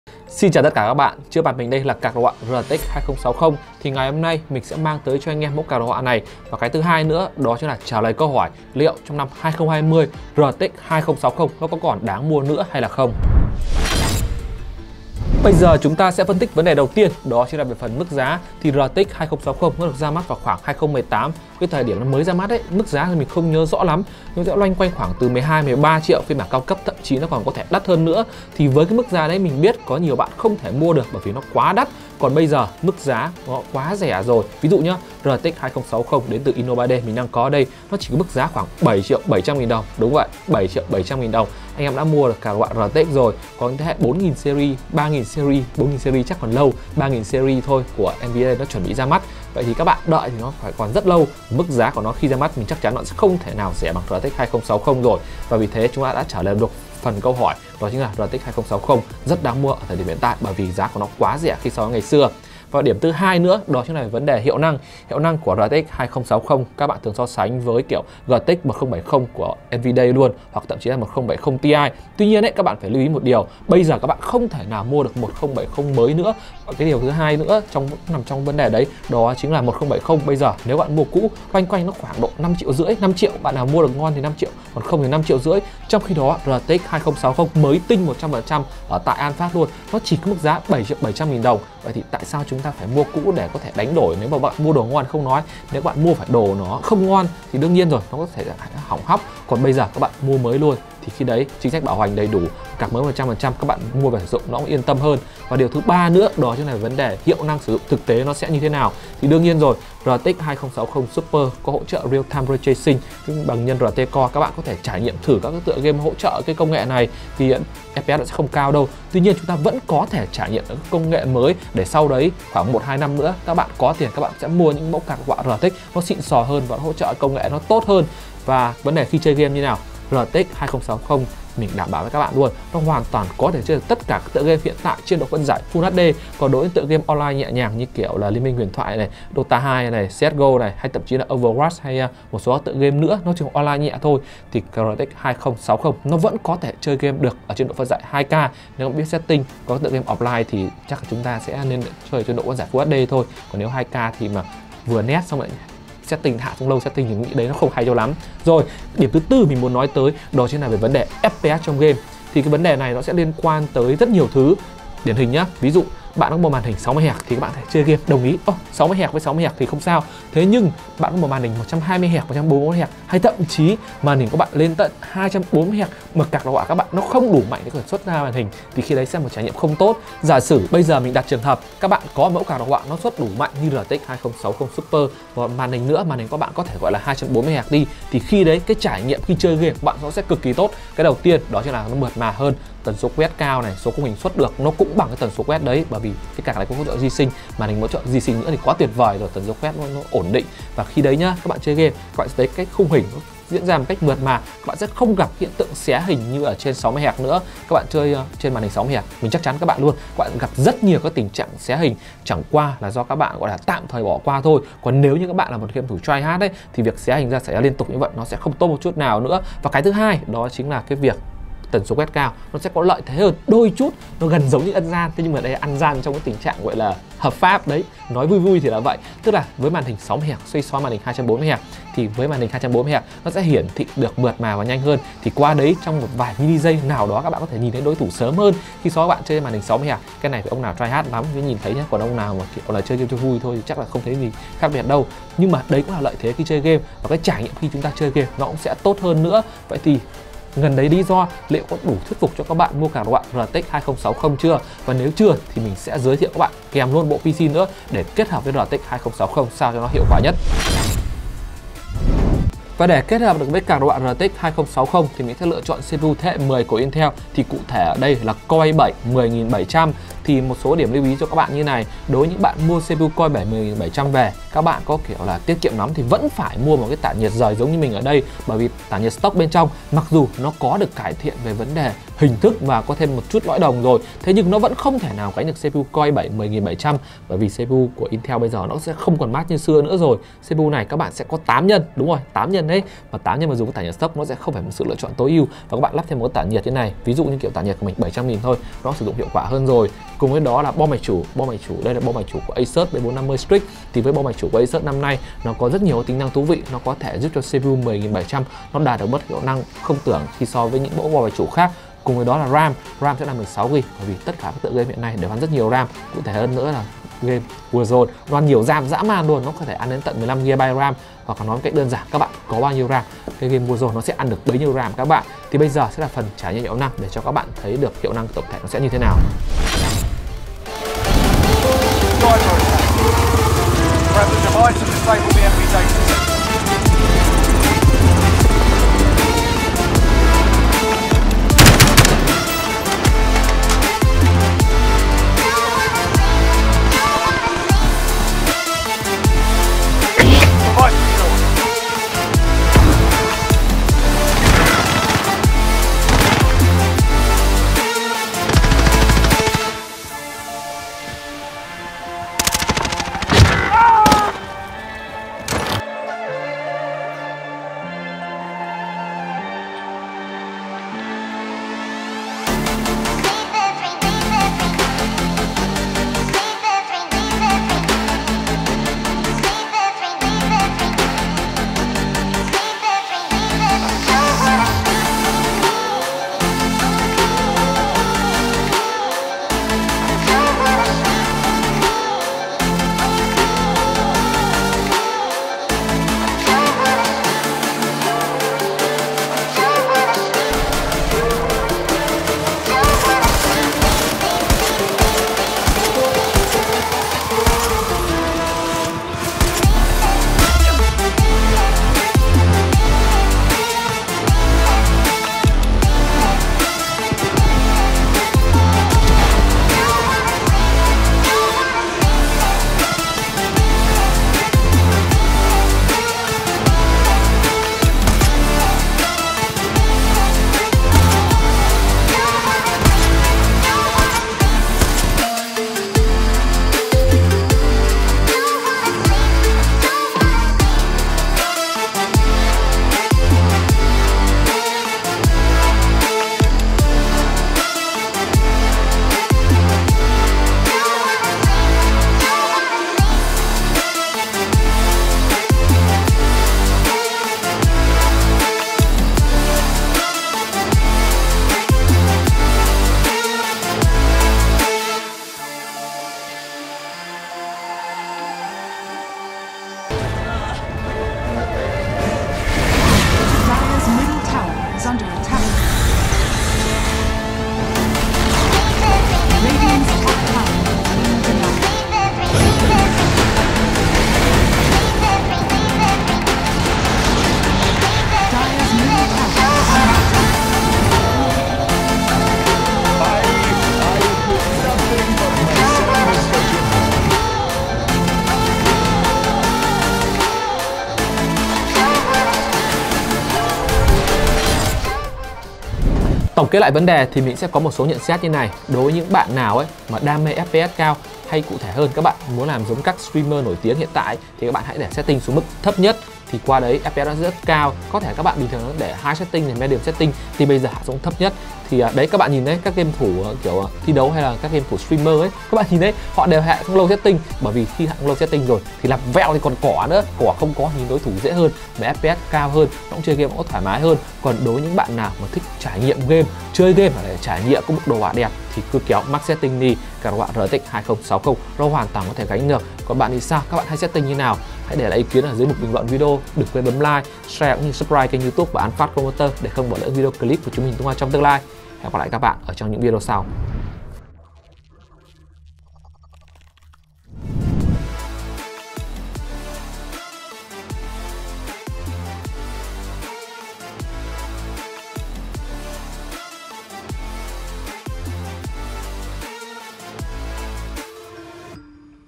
RTX 2060 liệu có còn đáng mua trong năm 2020 hay không ???| An Phat PC
$ Giá của em nó: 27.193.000 Đồng
🔹CPU Intel Core i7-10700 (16M Cache, 2.90 GHz up to 4.80 GHz, 8C16T, Socket 1200, Comet Lake-S)
🔹Mainboard ASUS ROG STRIX B460-G GAMING
🔹Ram PNY XLR8 Gaming 8GB (2x8GB) DDR4 3200MHz
🔹VGA INNO3D GEFORCE RTX 2060 GAMING OC X2 6GB
🔹SSD PNY XLR8 Gaming CS3030 250GB NVMe M.2 2280 PCIe Gen 3.0 x4
🔹Nguồn máy tính Cooler Master Elite V3 230V PC600 600w
🔹Vỏ case ANTEC One Hundred AP workstation + 2 fan điều tốc Hoặc Vỏ case GAMEMAX REVOLT
🔹Tản nhiệt khí ID-Cooling CPU SE-224-XT ARGB
►Lịch Live:
* Tư vấn miễn phí: 16:00-17:00 hàng ngày

📌Nếu anh em có điều gì thắc mắc đừng quên để lại COMMENT ở phía dưới nhé.